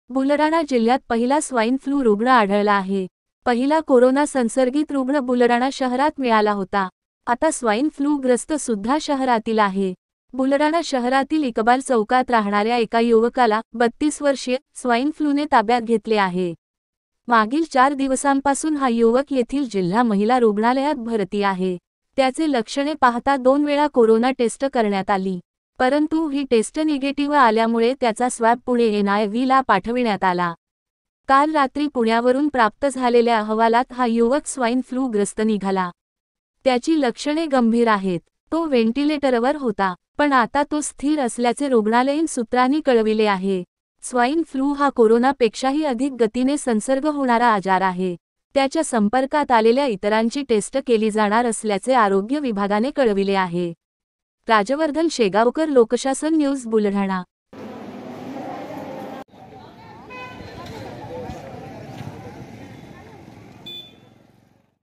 Buldhana I बुलढाण्यात स्वाईन फ्लू ची एंट्री,बुलढाणा शहरातील इक्बाल चौकातील एक युवक पॉझिटिव्ह
राजवर्धन शेगावकर बुलढाणा
बुलडाणा जिल्ह्यात पहिला स्वाईन फ्लू रुग्ण आढळला आहे. पहिला कोरोना संसर्गीत रूग्ण बुलडाणा शहरात मिळाला होता. आता स्वाईन फ्लू ग्रस्त सुद्धा शहरातील आहे. बुलडाणा शहरातील इकबाल चौकात राहणाऱ्या एका युवकाला (32 वर्षीय) स्वाईन फ्लूने ताब्यात घेतले आहे.
मागील 4 दिवसांपासून हा युवक येथील जिल्हा महिला रुग्णालयात भरती आहे. त्याचे लक्षणे पाहता दोन वेळा कोरोना टेस्ट करण्यात आली. परंतु ही टेस्ट निगेटिव्ह आल्यामुळे त्याचा स्वॅब पुणे एनआयव्ही ला पाठविण्यात आला. काल रात्री पुण्यावरून प्राप्त झालेल्या अहवालात हा युवक स्वाईन फ्लू ग्रस्त निघाला. त्याची लक्षणे गंभीर आहेत. तो व्हेंटिलेटरवर होता. पण आता तो स्थिर असल्याचे रुग्णालयीन सूत्रानी कळविले आहे. स्वाईन फ्लू हा कोरोना पेक्षाही अधिक गतीने संसर्ग होणारा आजार आहे. त्याच्या संपर्कात आलेल्या इतरांची टेस्ट केली जाणार असल्याचे आरोग्य विभागाने कळविले आहे.